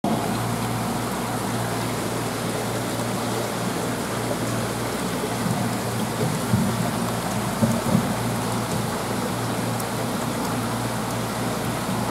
小魚を水槽へ入れると
クエが捕食しました。